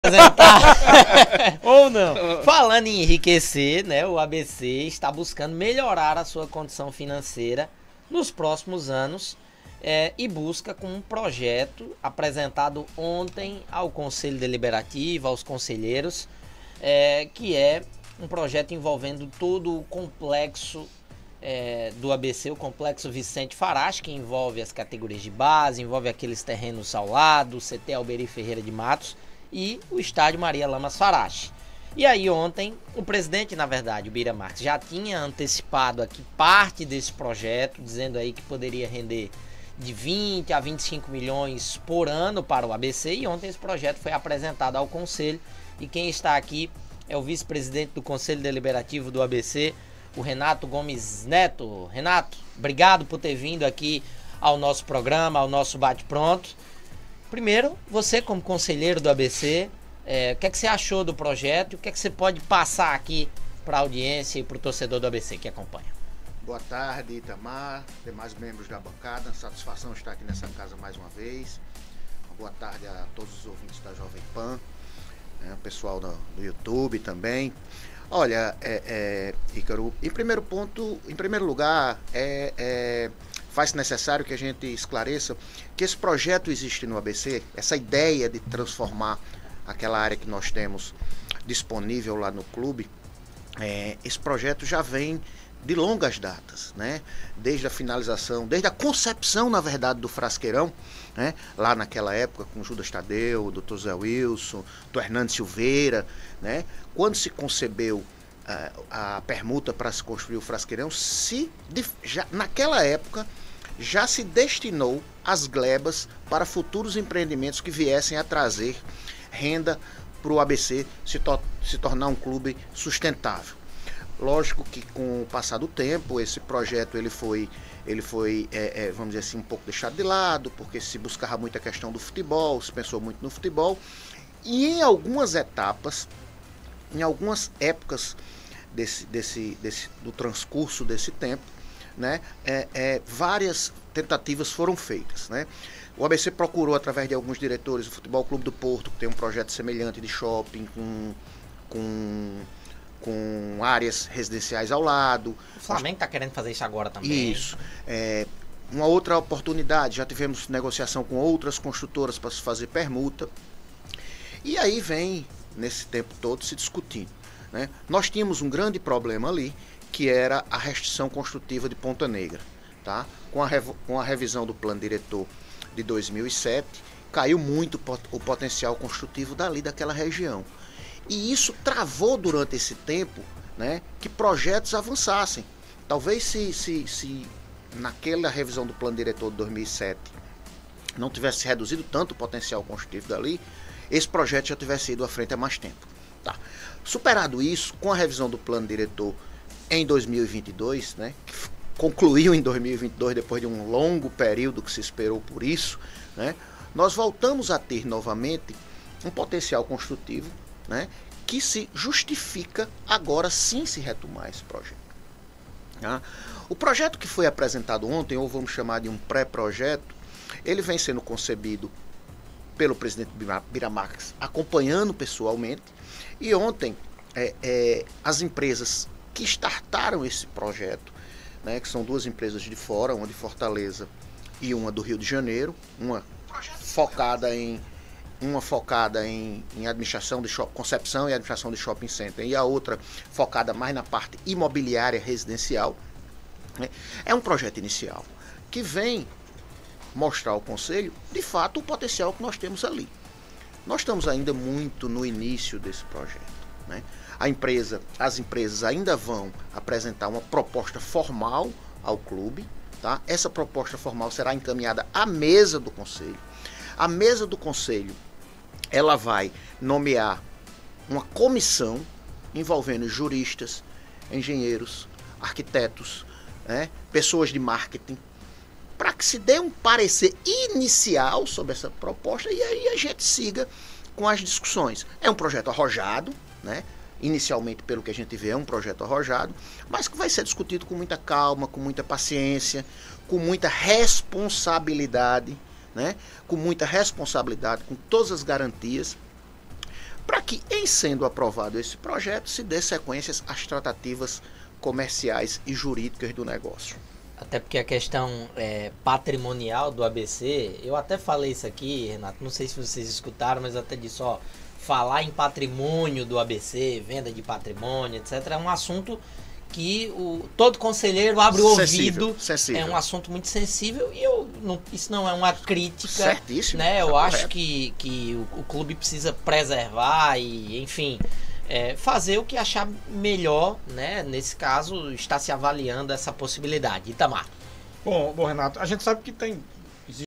"REVISÃO do Plano Diretor de Natal foi BENÉFICA para o ABC! O que foi APRESENTADO aos conselheiros f
/ jovempannewsnatal